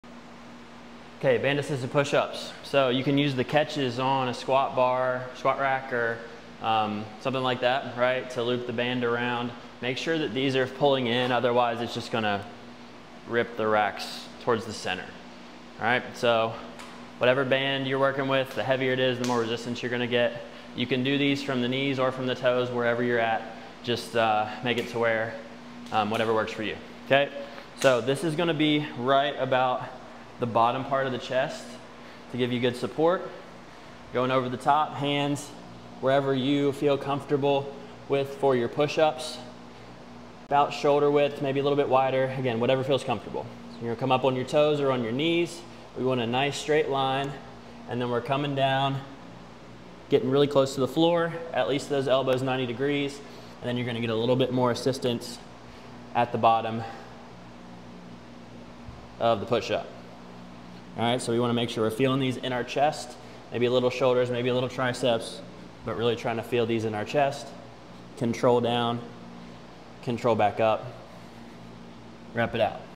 Band Assisted Push Up
-note where you put the catches so you can keep it consistent every time
-place band at the lower part of your chest
-hands a little wider than shoulder width or what is comfortable for you
-keep core braced and straight line from shoulder to knee/ankle throughout the exercise
-use your chest